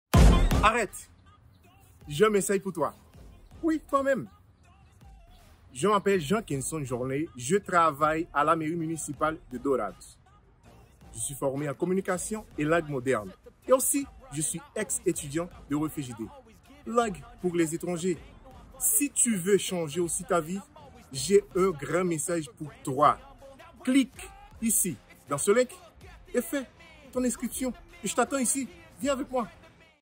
Curso de Português para Estrangeiros
O Centro de Formação da Universidade Federal da Grande Dourados (UFGD) está com inscrições abertas para o Curso de Português para Estrangeiros Refugiados, imigrantes ou portadores de visto humanitário poderão inscrever-se gratuitamente a partir de 25 de abril de 2022. O Curso de Português para Estrangeiros – Módulo Acolhimento objetiva proporcionar uma aprendizagem rápida dos elementos mais urgentes para uma inserção linguística, social e laboral do aprendiz.